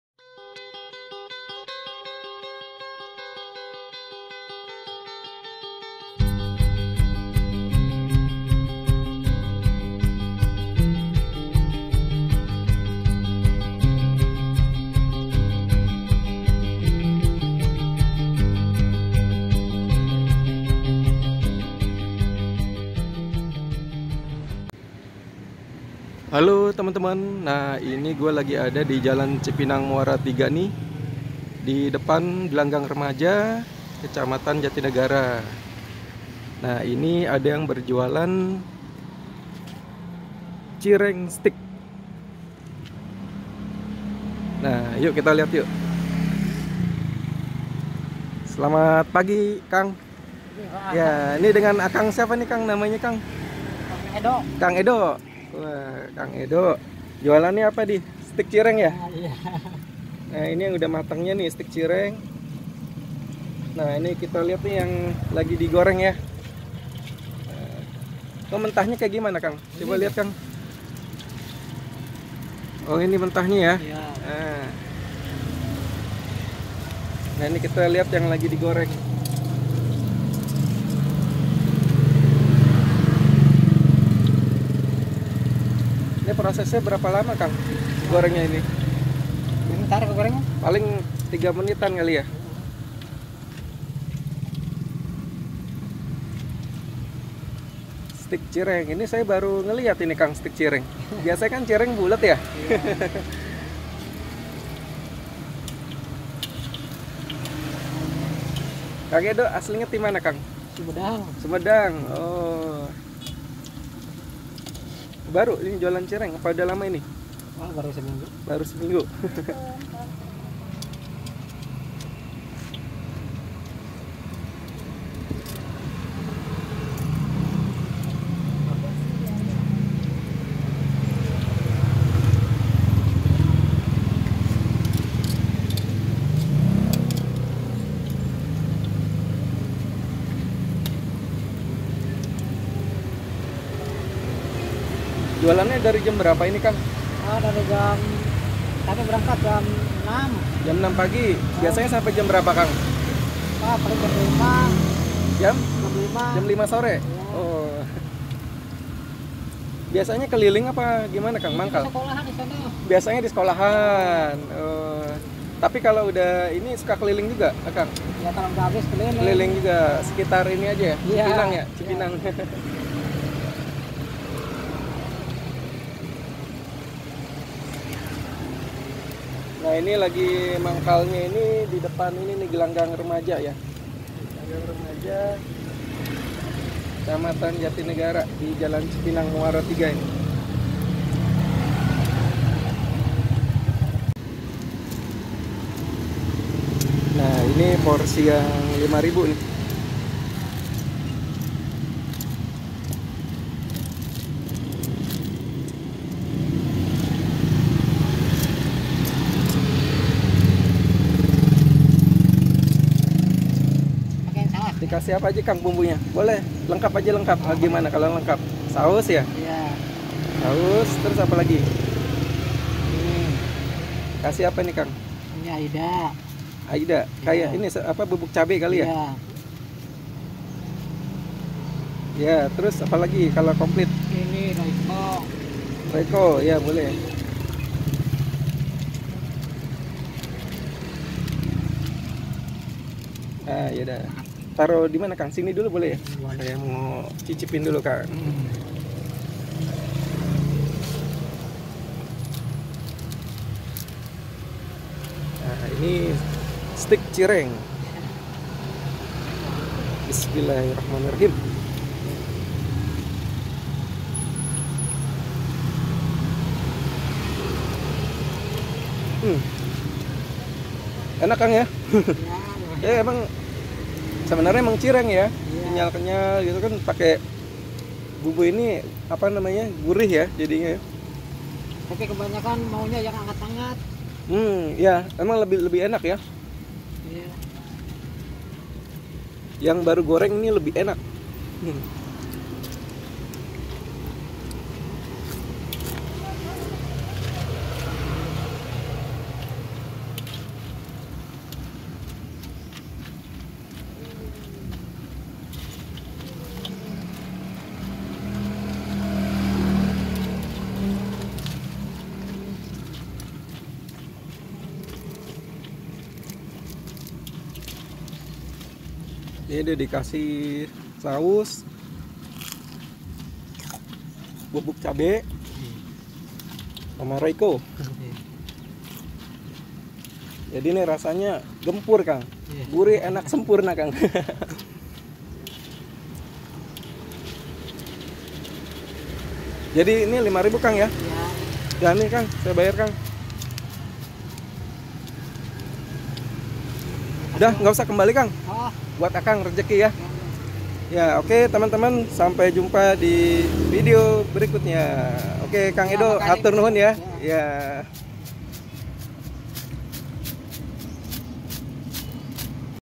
IDE JUALAN!CIRENG STIK KANG EDO CEMILAN UNIK GURIH, ENAK DAN MURAH.
Cireng stik bisa beli 2rb, 3rb, 5rb

Jualannya di Jl.Cipinang Muara III biasanya mangkal di depan SD 16 - 17 Pagi Cipinang Muara III, Jak-Tim.
Waktu sy liput posisinya lg dedepan Gelanggang Remaja Cipinang Muara III.

Jualan dari jam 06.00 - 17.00 WIB

Selamat Menonton!
Salam Gempur (Gurih, enak, sempurna)